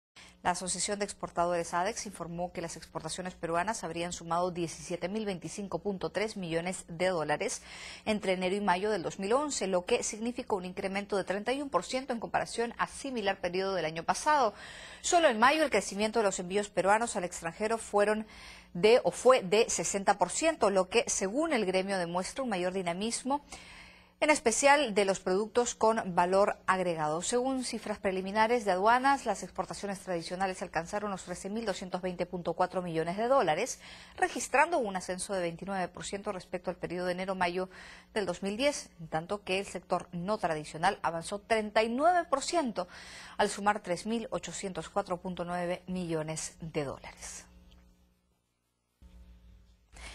Exportaciones peruanas habrían crecido 31% a mayo de este año
Resumen económico
Locución en Off

ADEX informó que las exportaciones alcanzaron unos 19 mil 25.3 millones de dólares entre enero y mayo del 2011, lo que significa un incremento de 31% en el mismo lapso de tiempo del año pasado.

Fuente: CANAL 8 / RUMBO ECONÓMICO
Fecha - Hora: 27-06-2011